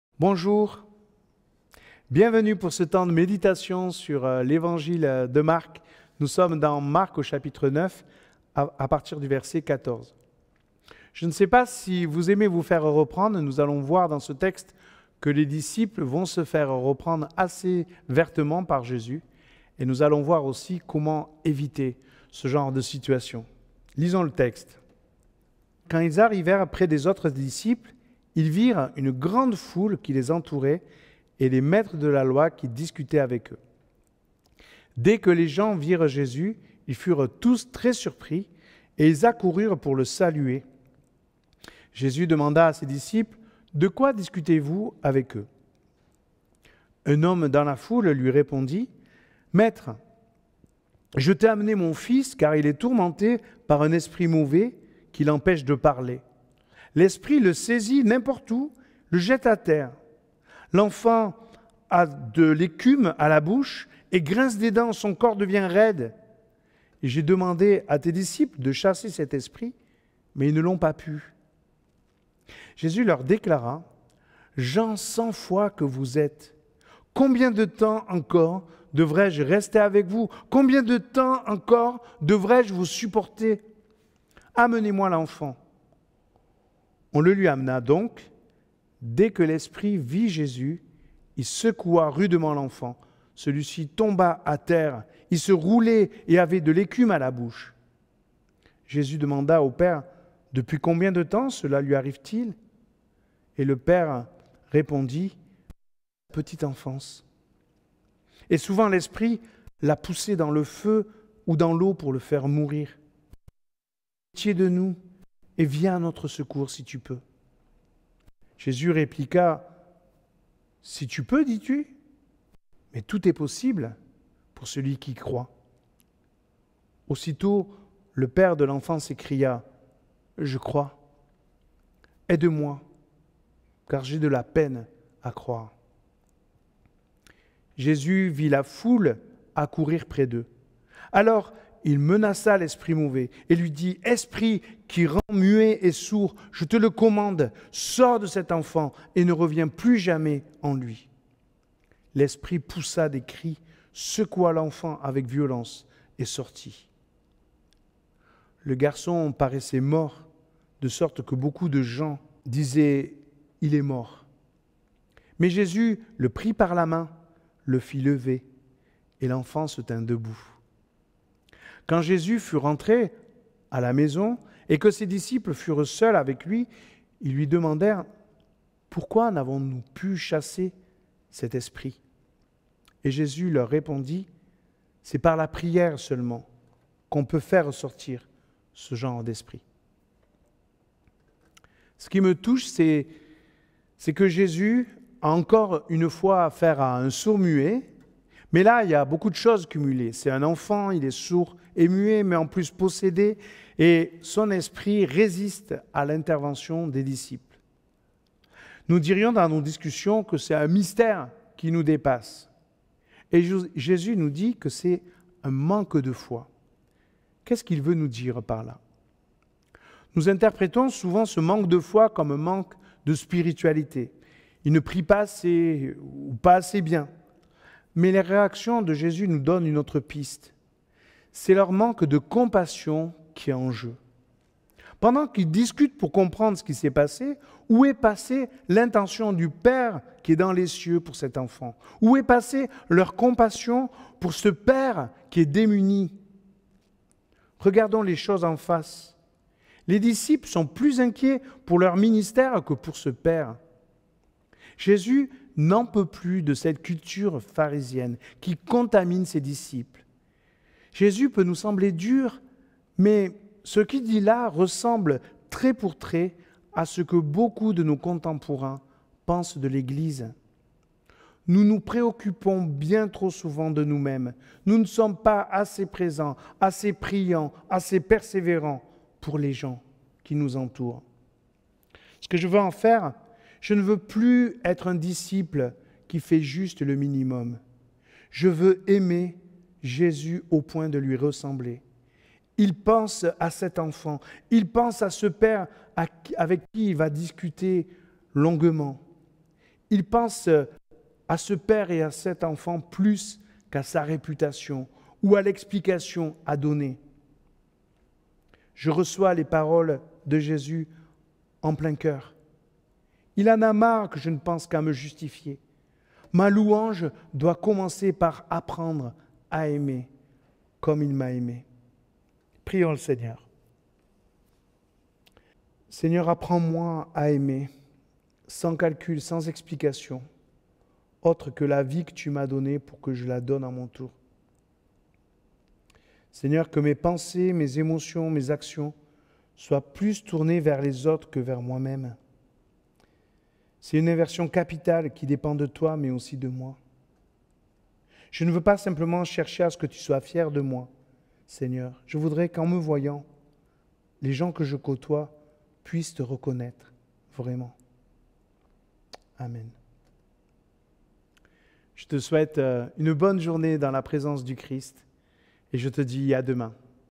Vivre 28 - Recevoir les paroles de Jésus en plein cœur - Jean-Pierre Civelli - Église M
Recevoir les paroles de Jésus en plein cœur

Lecture : Évangile de Marc 8.14-21
Jeudi 23 avril 2020
Méditation apportée par Jean-Pierre Civelli

Ce n’est pas la première fois que nous avons affaire à un sourd-muet mais là tout est cumulé : c’est un enfant, sourd et muet mais en plus possédé et résistant à l’intervention des disciples... Nous dirions dans nos discussions que c’est un mystère qui nous dépasse.
Jésus nous dit que c’est un manque de foi ! Que veut-il dire par là ?
Nous interprétons souvent ce « manque de foi » comme un manque de spiritualité : ils ne prient pas assez, ou pas assez bien... Mais la réaction de Jésus nous donne une autre piste : c’est leur manque d’amour qui est en jeu !
Pendant qu’ils discutent pour comprendre ce qui s’est passé, où est passée l’intention du Père dans les cieux pour cet enfant, où est passée leur compassion pour ce père sur terre, démuni ? Regardons les choses en face : les disciples sont plus inquiets pour leurs ministères que pour ce père !
Jésus n’en peut plus de cette « culture pharisienne » qui contamine ses propres disciples. Jésus nous semble dur ? Ce qu’il dit là ressemble trait pour trait à ce que beaucoup de nos contemporains pensent de l’église !
Nous nous préoccupons bien trop souvent de nous-mêmes, nous ne sommes pas assez pré- sents pour les gens, dont acte !